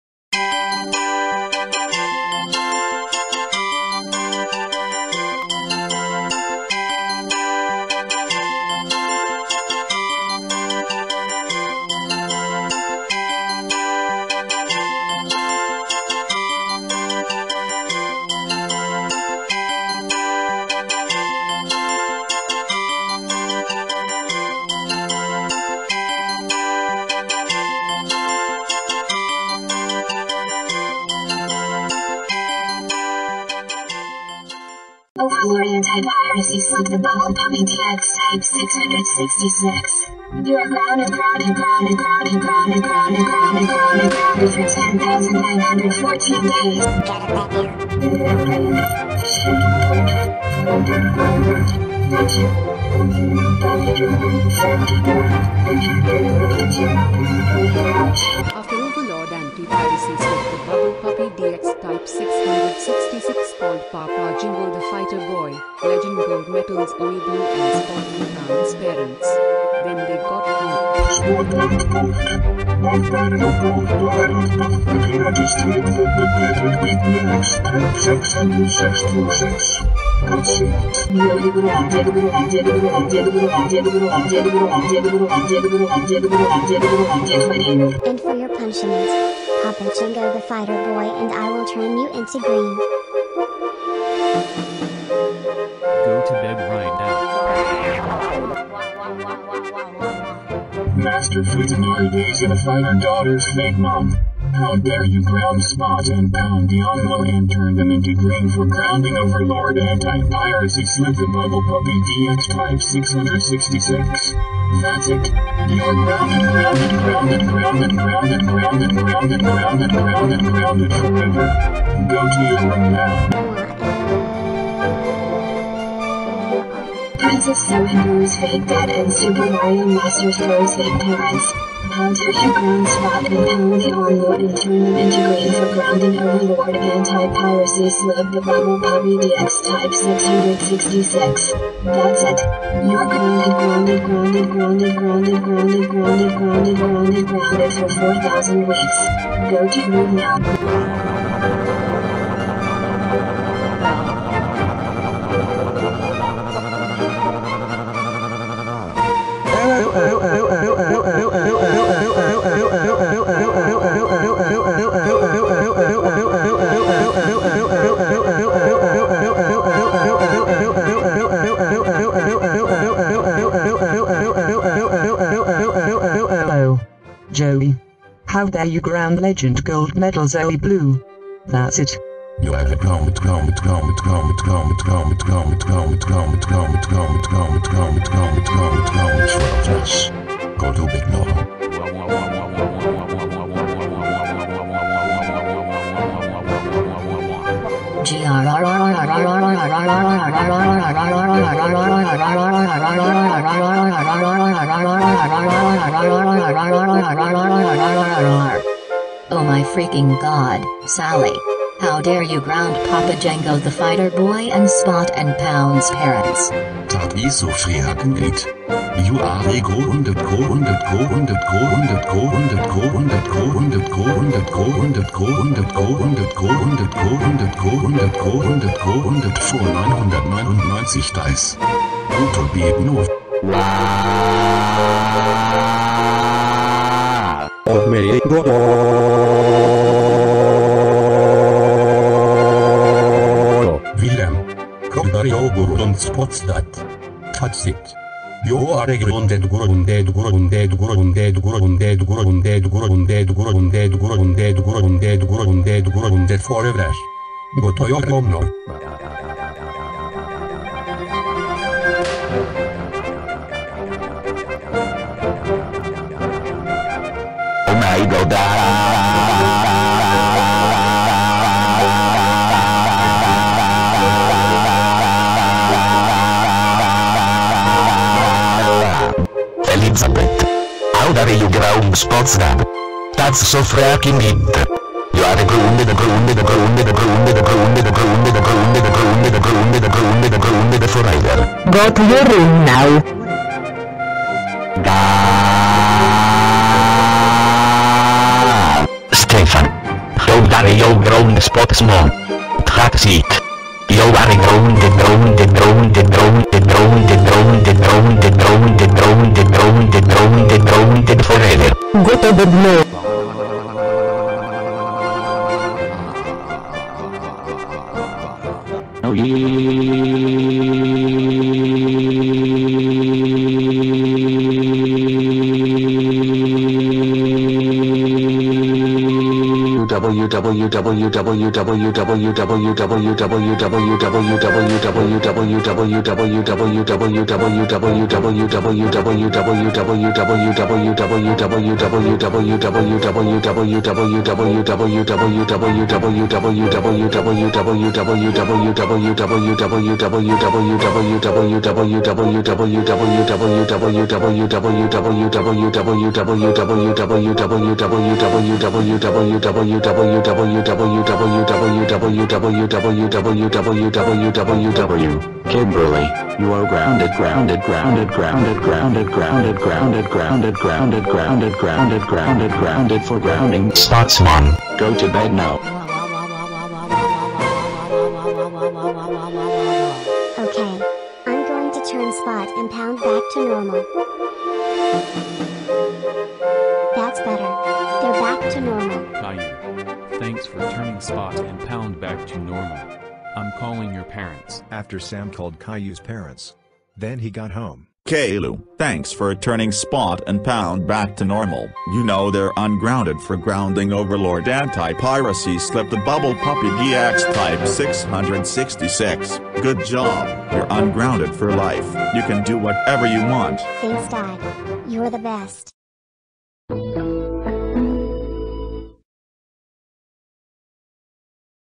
Spot's Playhouse EP8 Spot And Pound Got Turned Into Green
Spot And Pound got turned into green But Caillou had to turn Spot And Pound Back to normal!